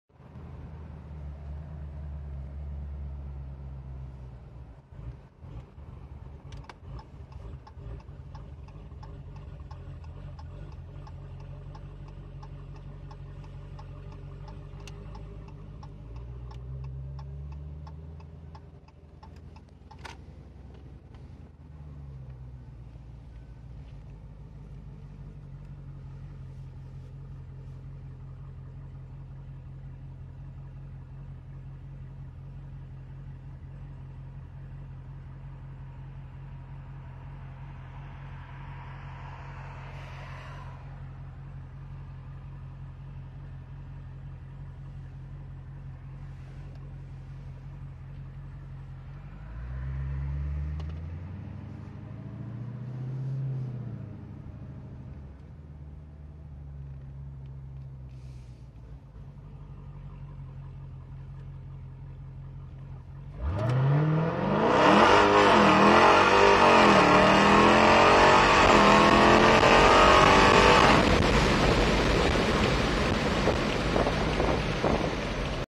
2018 Hellcat Widebody WOT AFR
Shooting some AFR on my 2018 WB Hellcat.